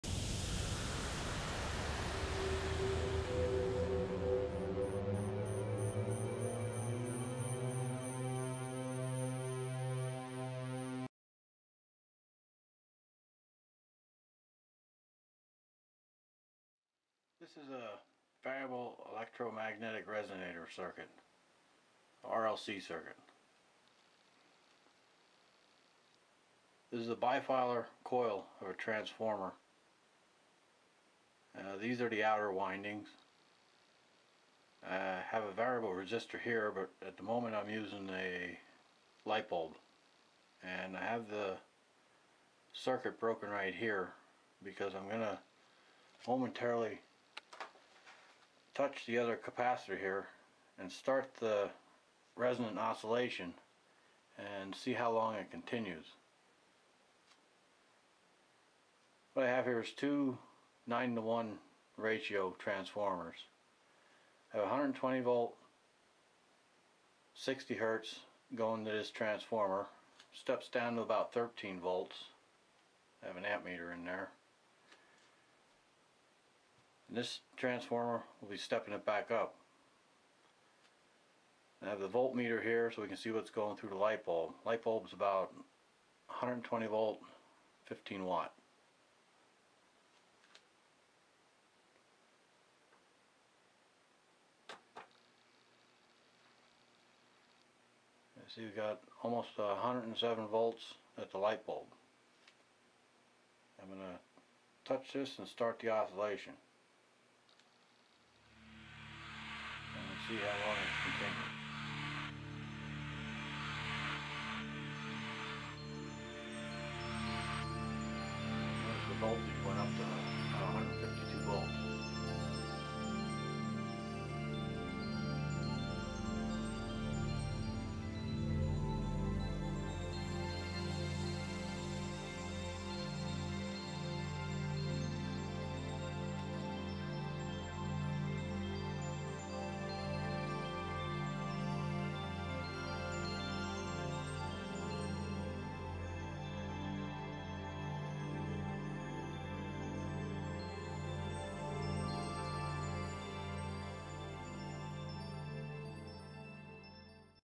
Variable Electromagnetic Resonator - Ring Time
This circuit should easily tune a joint or magnetically coupled circuit to resonance.